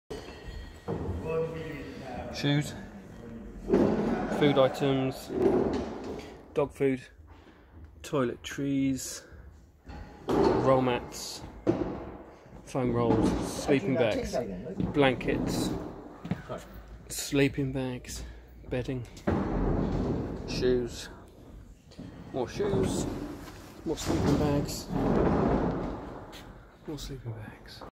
Warm clothing items for the homeless
These are the items we've (The Hedgehogs Farnham) collected for our homeless appeal.